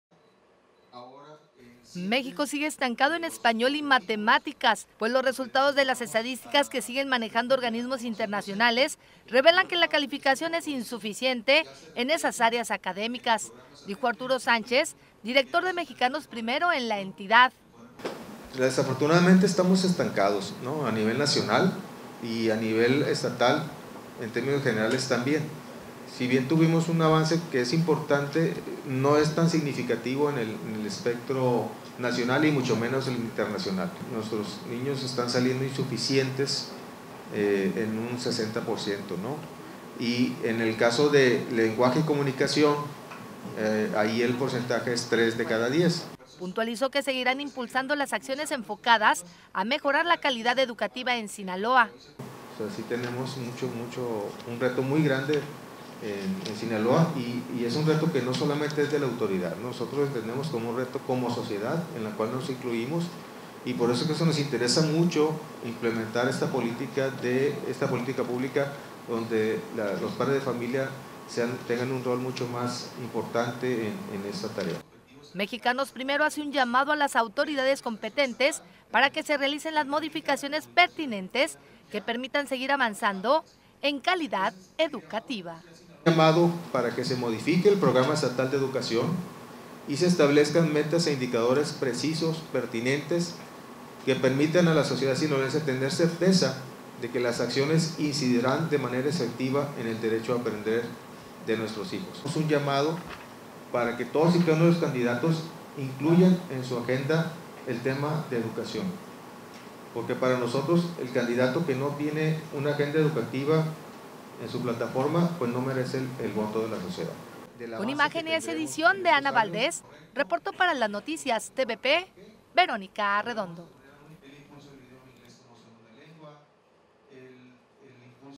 Seguimos reprobados
México, sigue estancado en Español y Matemáticas, pues los resultados de las estadísticas que siguen manejando organismos internacionales, revelan que la calificación es insuficiente en esas áreas académicas, dijo Arturo Sánchez, Director de Mexicanos Primero en la Entidad.
Puntualizó que seguirán impulsando las acciones enfocadas a mejorar la calidad educativa en Sinaloa.
Mexicanos primero, hacen un llamado a las autoridades competentes para que se realicen las modificaciones pertinentes que permitan seguir avanzando en la calidad educativas.